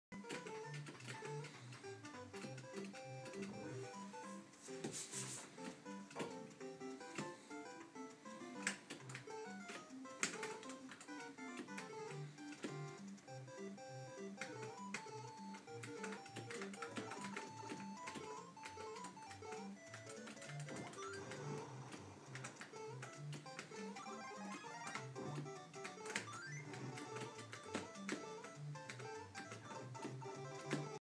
Testing the control panel for the first time
Zac tests our HyperSpin Arcade's control panel for the first time.